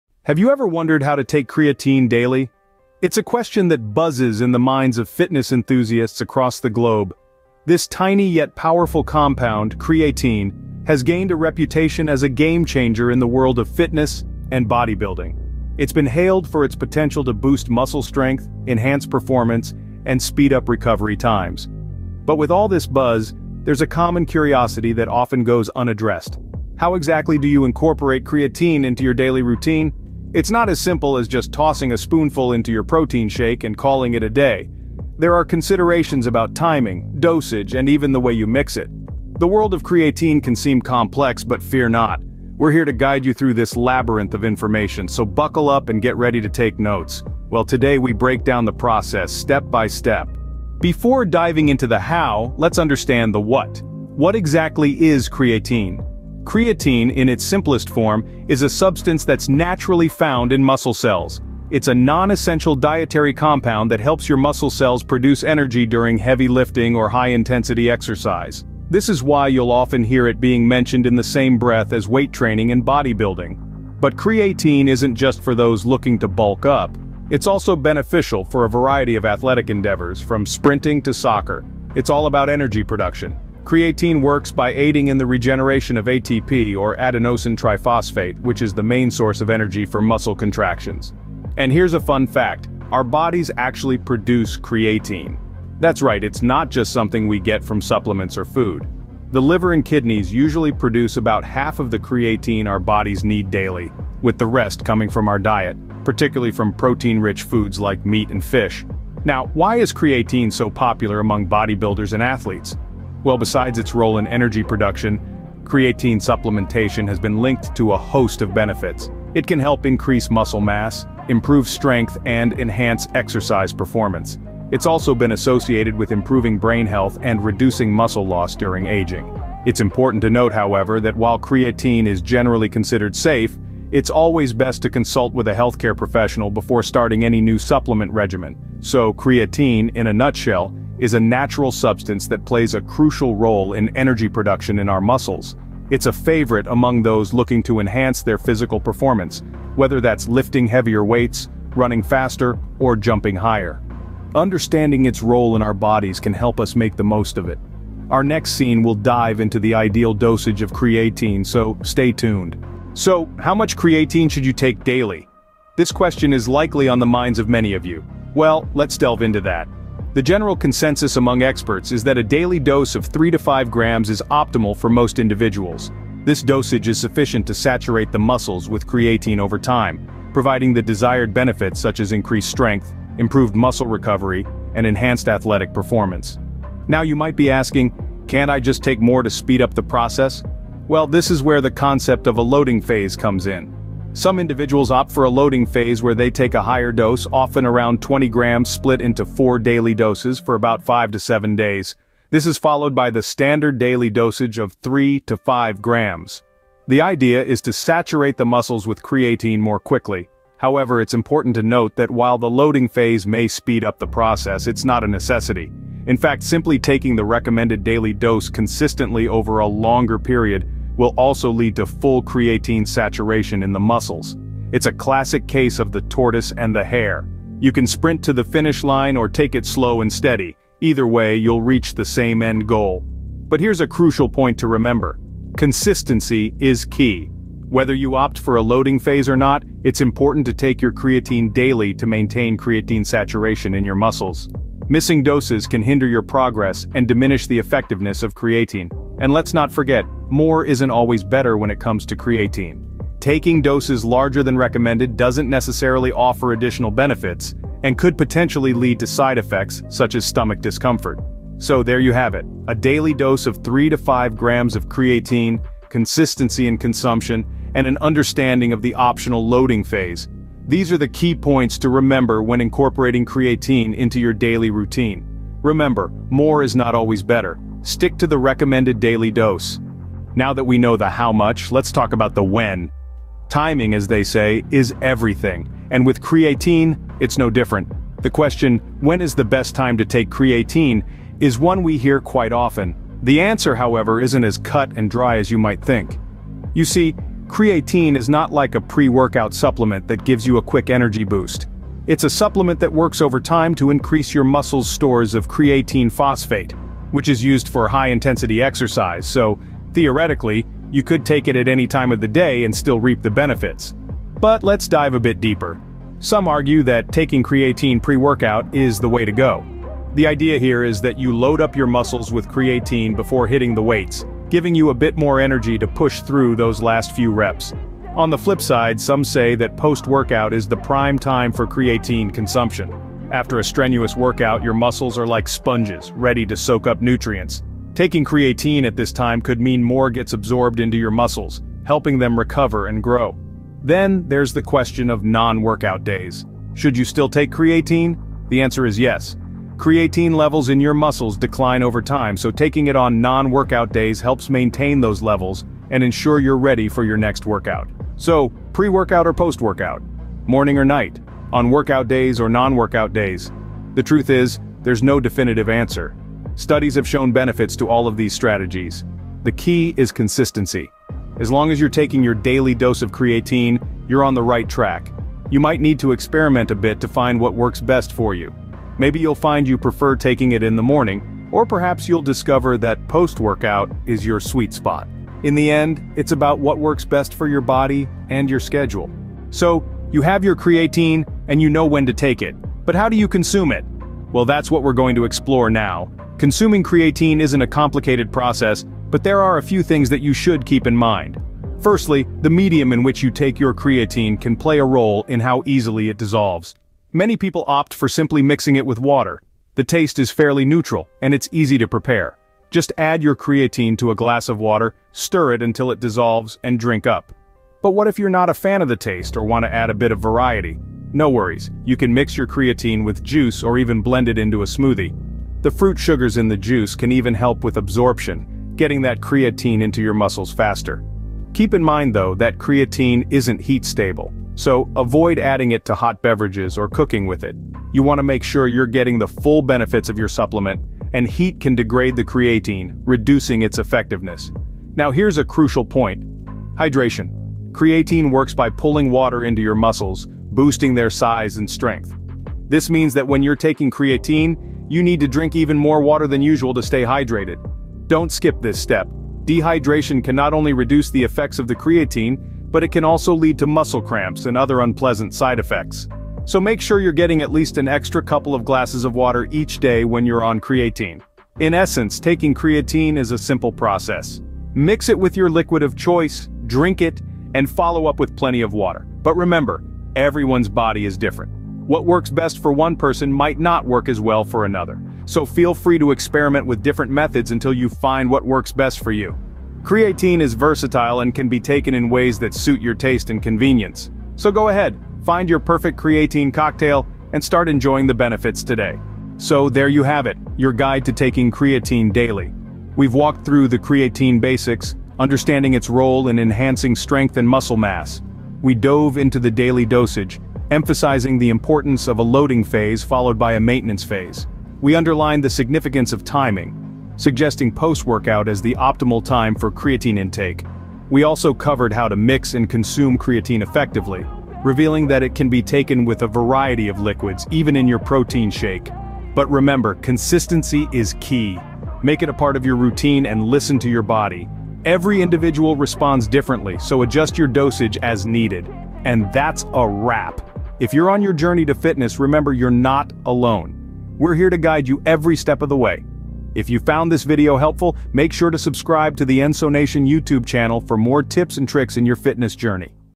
how to take creatine for beginners
Video Description:

Welcome to the Nsonation YouTube channel, where we provide valuable insights on fitness and nutrition! In this video, we dive into the world of creatine and how it can enhance your fitness journey.

Are you unsure about the correct way to take creatine daily? Look no further! Our expert guide will walk you through everything you need to know, from dosages to timing and even potential side effects.

Taking creatine can be a game-changing addition to your workout routine, so make sure to stick around till the end for some bonus tips and tricks.

OUTLINE:

00:00:00 The Creatine Curiosity
00:00:50 The Creatine Basics
00:02:42 Daily Dosage
00:04:40 Timing is Everything
00:06:29 Mixing and Consuming Creatine
00:08:19 Key Takeaways and Call to Actioncreatine monohydrate#how to take creatine daily#testosterone#begbeginners guide